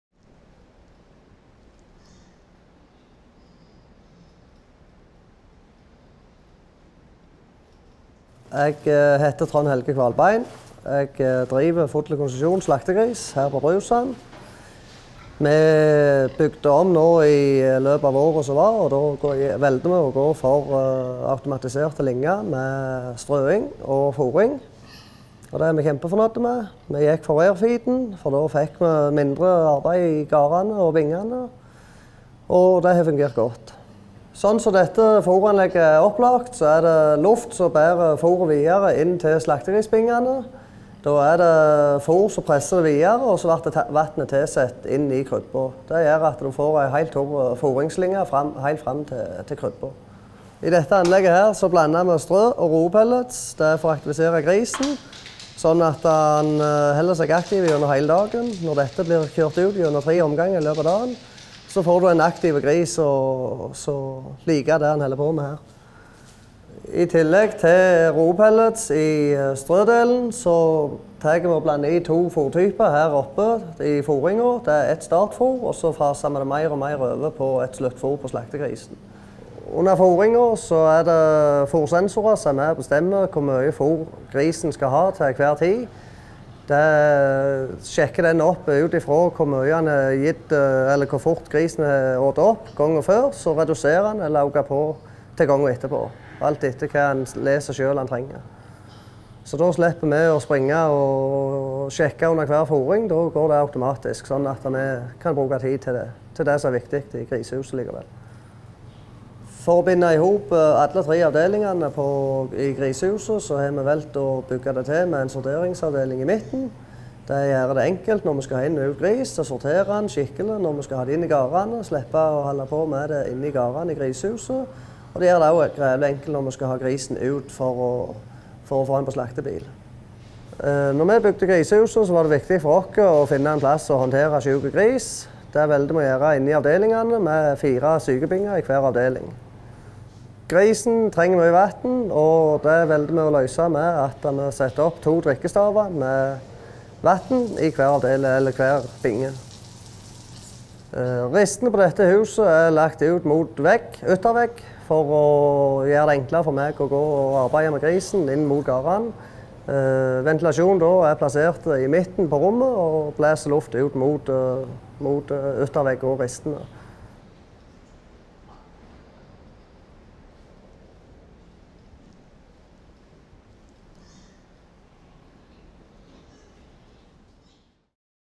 Innovative løsninger i fremtidens grisefjøs
Se hvordan den automatiserte strøingen og fôringen i kombinasjon med AirFeed, foregår hos Trond Helge Qvalbein, som driver full konsesjon slaktegris på Brusand.